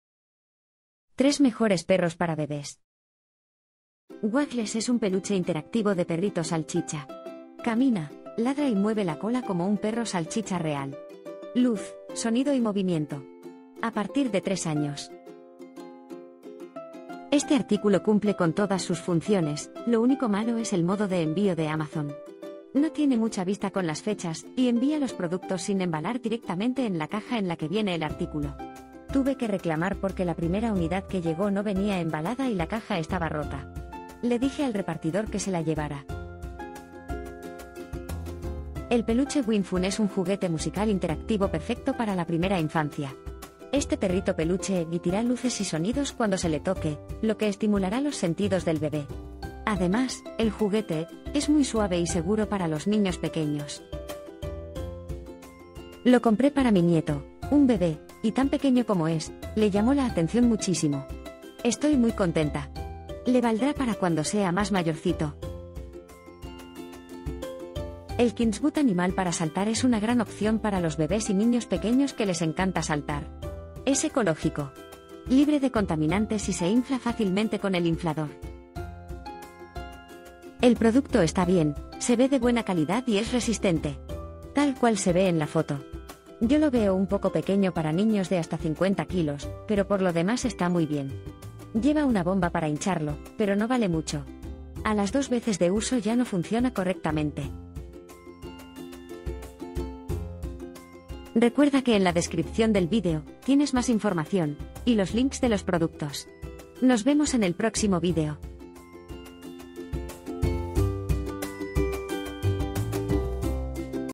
Perros para bebes: 3 Mejores Perros para Bebés
✋  Aquí están 3 mejores perros para bebés disponibles en el mercado.

??? ENLACES DE COMPRA PERROS PARA BEBES ???

1) Waggles, mi perrito salchicha. Peluche interactivo que camina, ladra y mueve la cola como un perro salchicha real. Luz, sonido y movimiento. A partir de 3 años.

2) winfun - Peluche luces y sonidos, Peluche musical, Primera infancia, Juguetes interactivos musicales, Peluche interactivo Perrito, Juguetes bebé 6 meses, Peluche bebé, Perrito peluche (85175)

3) Kindsgut Animal para Saltar, Animal de Salto, Saltar, Perro, brincar, para Bebes & niño pequeño, Inflable, con inflador, Unisex, ecológico/Libre de contaminantes

00:00 - Intro
00:04 - 1) Waggles, mi perrito salchicha. Peluche interactivo que...
00:38 - 2) winfun - Peluche luces y sonidos, Peluche musical,...
01:08 - 3) Kindsgut Animal para Saltar, Animal de Salto, Saltar,...
01:40 - Outro